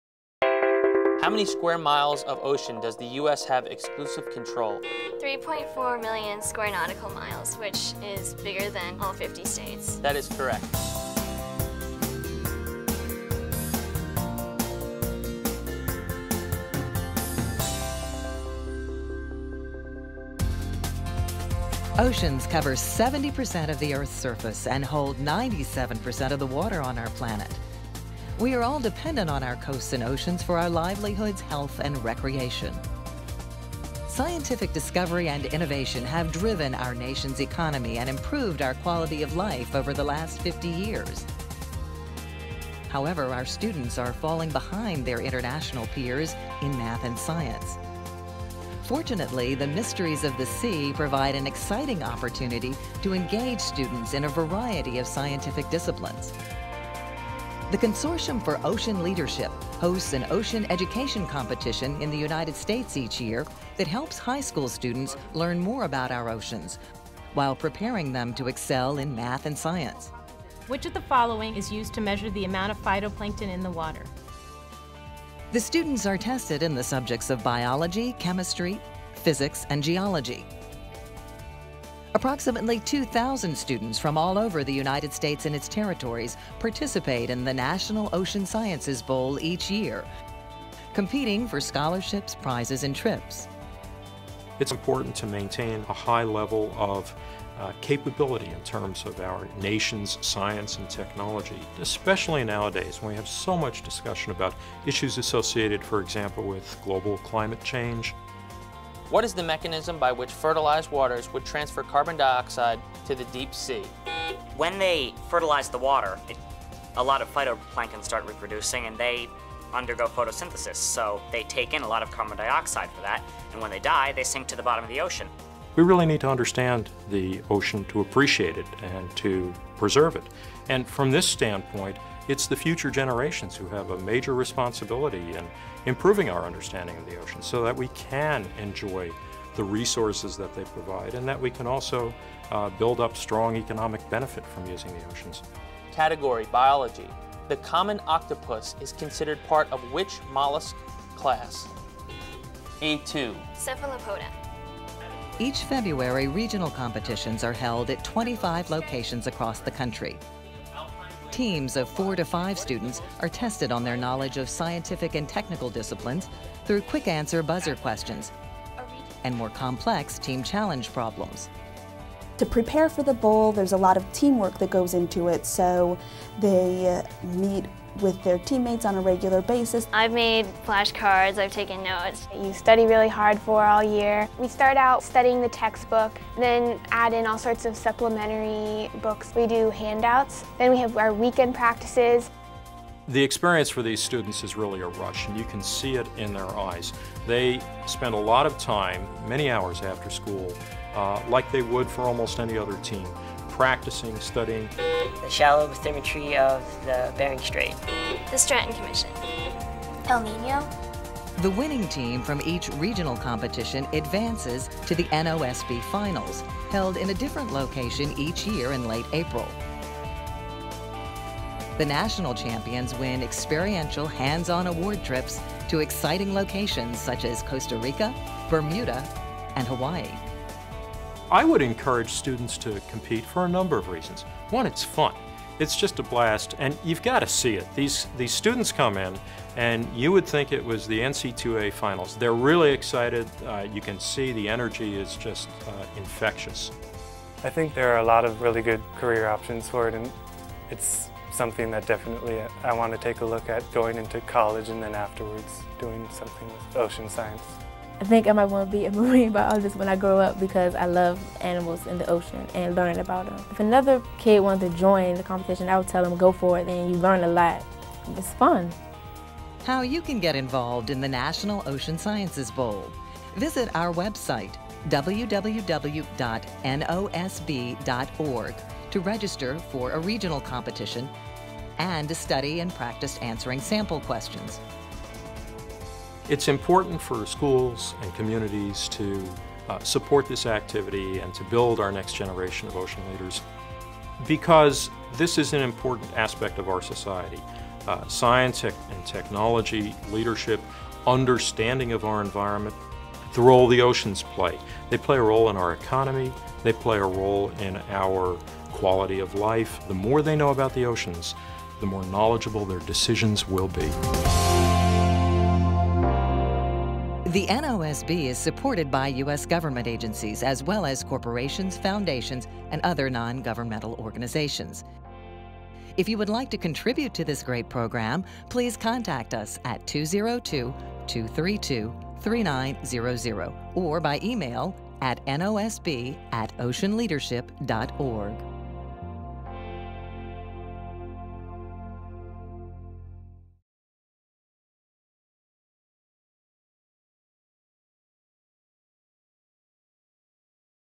NOSB Promotional Video (Full Version)
The National Ocean Sciences Bowl (NOSB®) is an academic competition that tests high
school students knowledge of the marine sciences including biology, chemistry, physics,
and geology. The NOSB was created in 1998 (The Year of the Ocean) to support and
expand ocean sciences education and enhance public understanding and stewardship of
the oceans. Each year approximately 2,000 students from 300 schools across the nation
compete for prizes and a trip to the national competition.